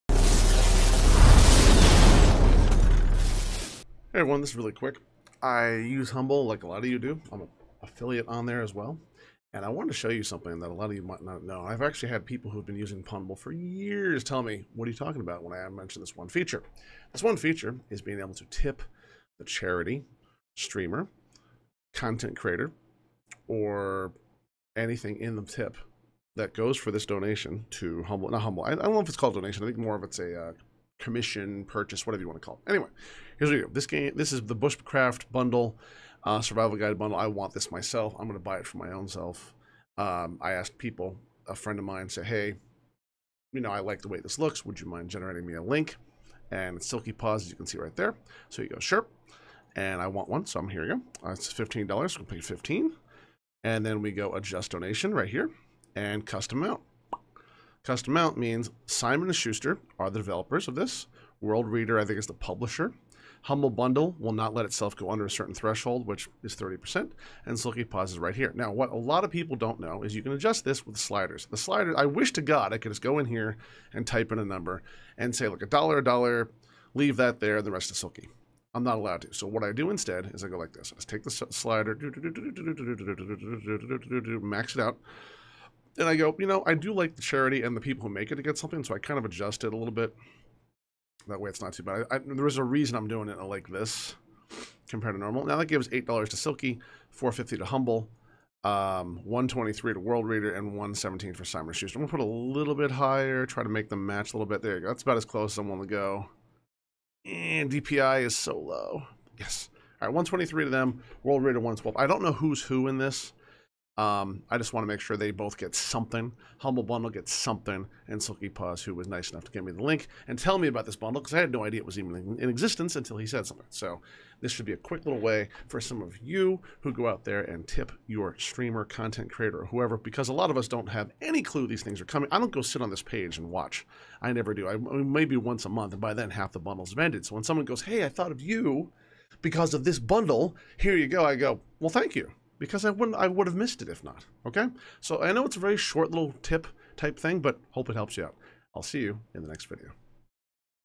Setting Custom tip amounts for YOUR affiliate when using Humble Bundle!
Want to know how to tip your Humble Affiliate?  Did you even think it was possible, IT IS!!!
And now I show you exactly how to do it and help your streamer, youtuber, blogger, etc when you use their link.
I know a ton of you will not use mine and that's okay!  I'm just glad to share the info, I don't always have to win and seeing a few other content folks be able to survive as we go into a recession, well that's just 'paying it forwards' in my book.
Any questions?
Put them below and I'll do my best to answer!